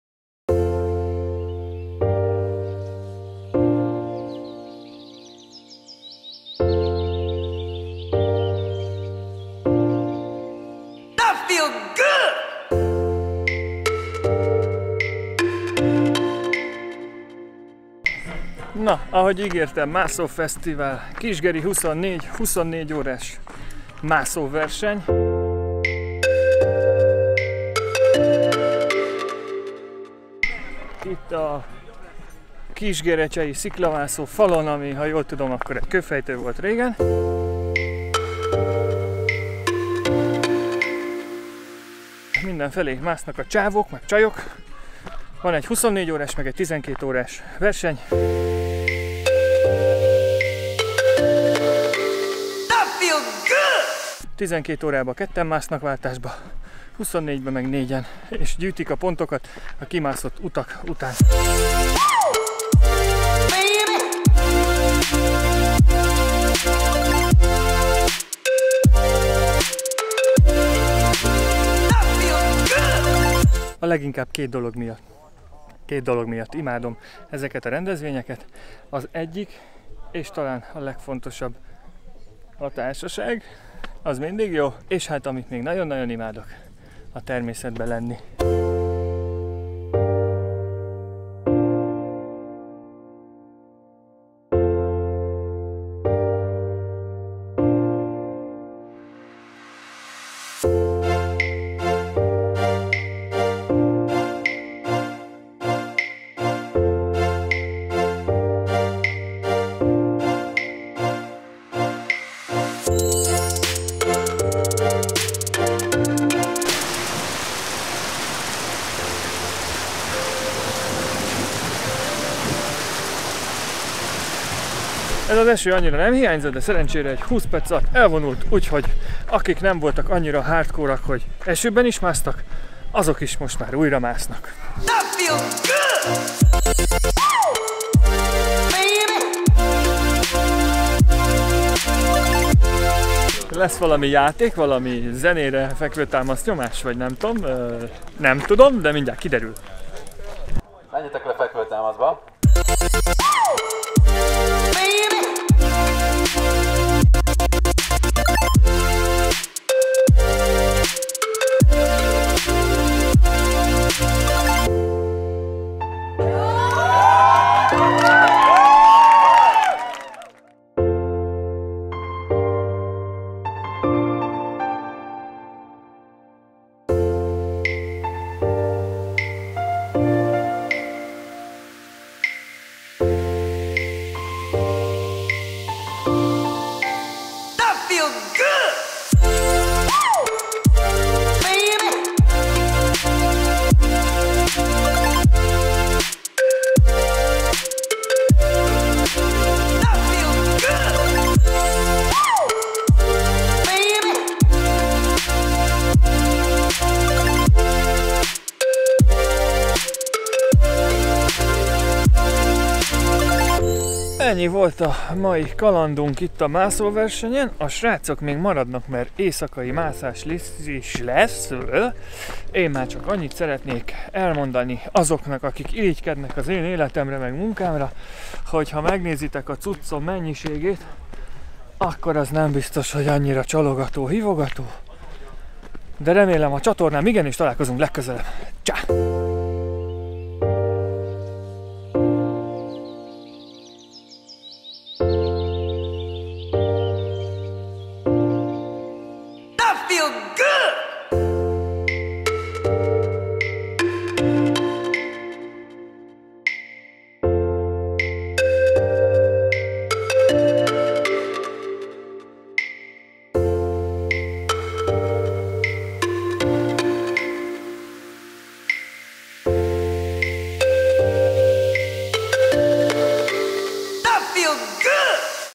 Sziklamászás - KisGeri 24 sziklamászó fesztivál és verseny
Elhívtak filmezni egy 24 órás sziklamászó fesztiválra, ahol nem mellesleg slacklineozni és highlineozni is lehetett. Sokáig nem kellett győzködni, imádom az outdoor sportokat, a társaság mindig jó, és a természetben eltöltött minden perc ajándék!
Sajnos a verseny maga nem volt igazán nyomon követhető, legalábbis számomra, de mindez az élményből semmit sem vett el, ezúton is gratulálok a győzteseknek!

Ha szeretnél Te is belekezdeni a filmezésbe, ajánlom a kezdő kurzusomat